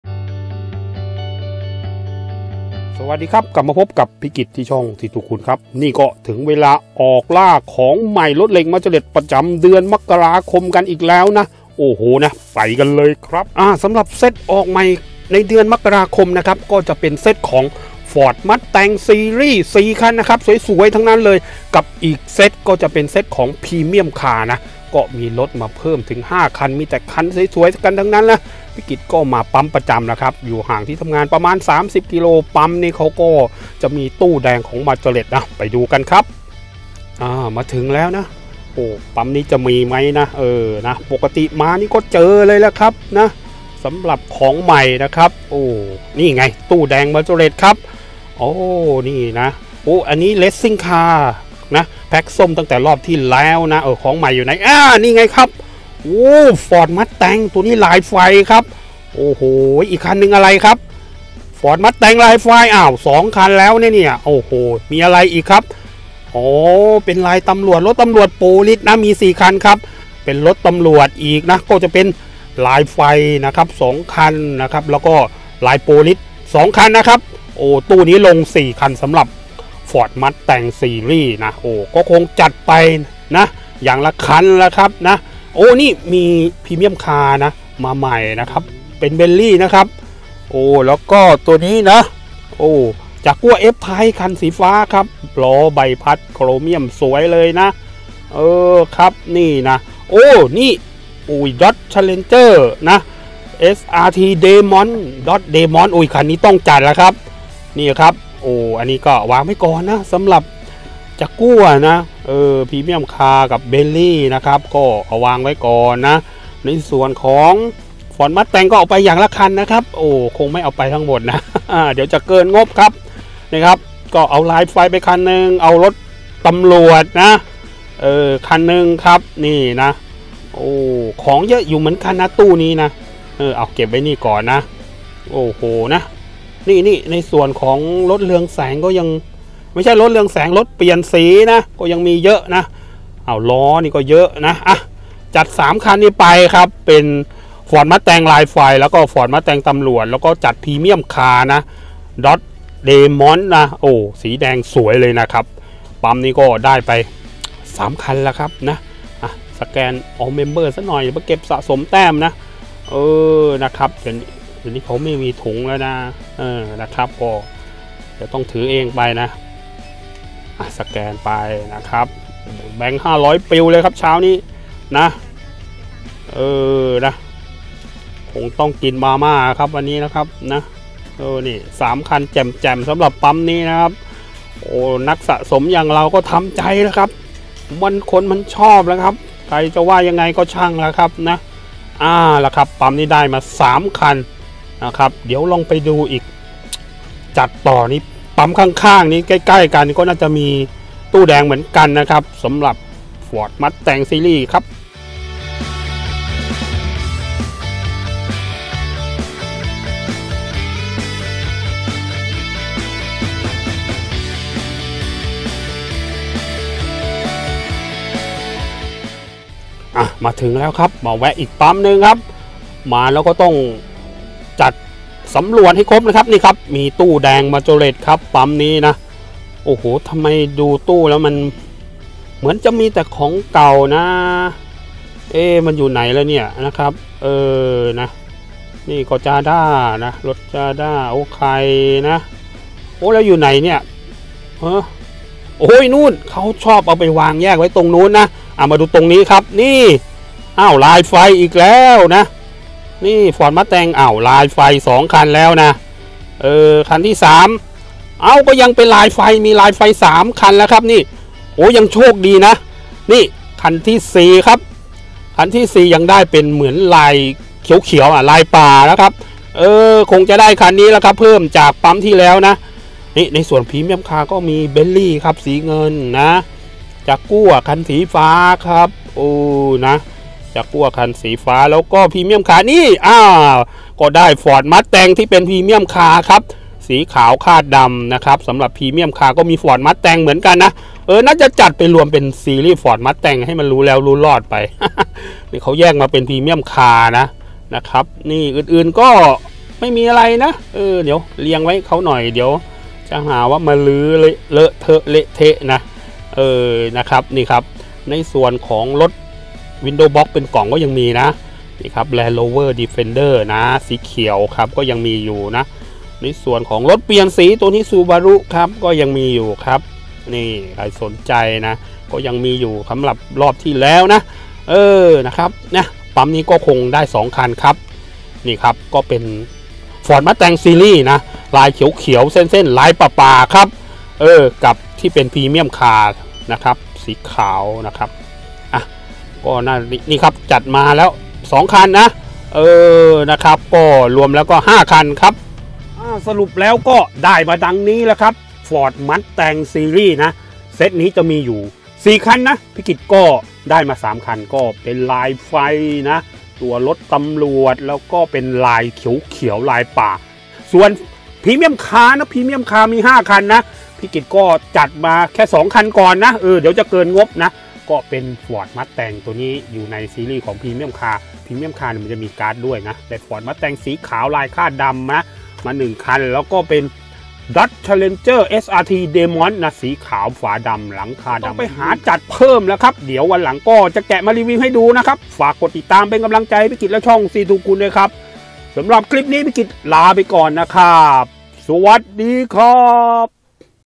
ล่ารถเหล็กออกใหม่มกราคม63| Ford Mustang Gt Series |C2Kun
รถเหล็กมาจอเร็ตชุดใหม่เดือนมกราคม63 ford mustanh gt series และ Premium car